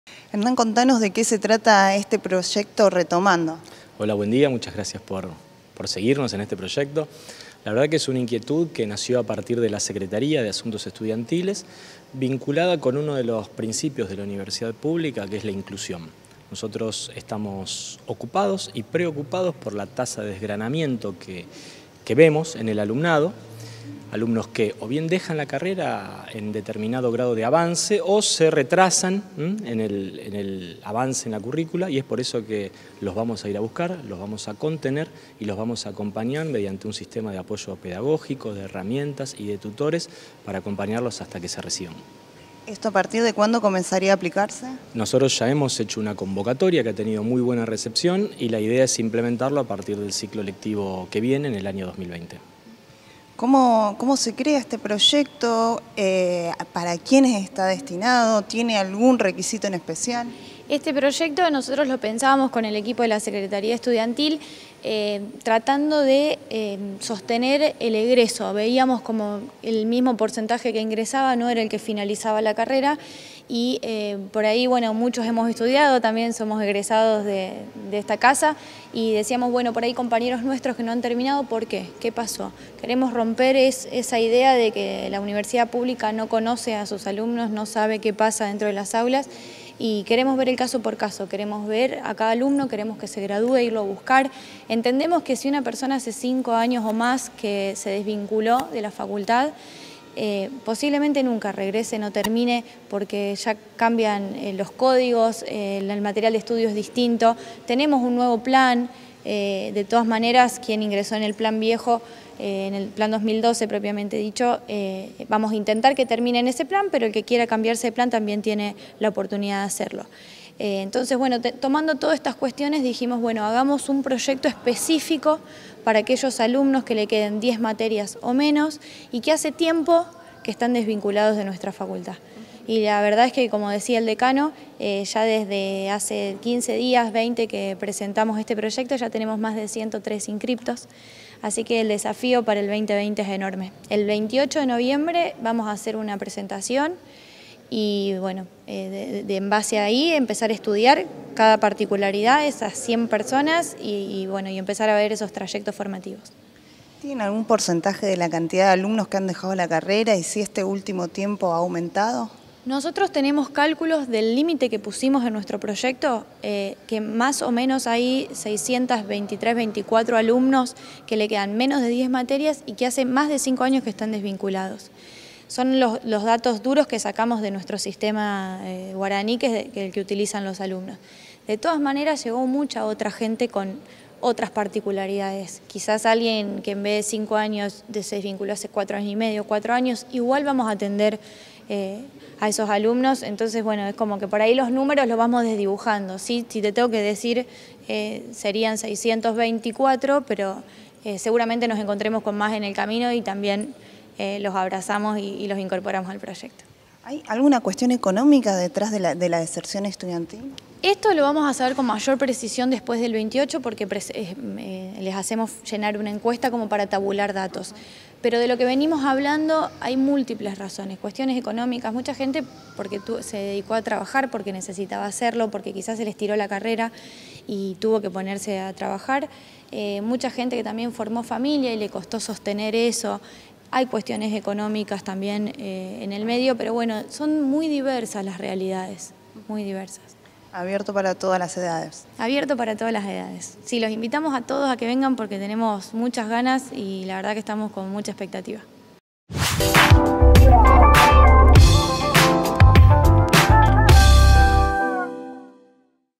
Presentan programa para que estudiantes de Derecho retomen y finalicen la carrera
Se trata de una iniciativa que arranca en 2020 y pretende que estudiantes que adeudan materias se puedan recibir.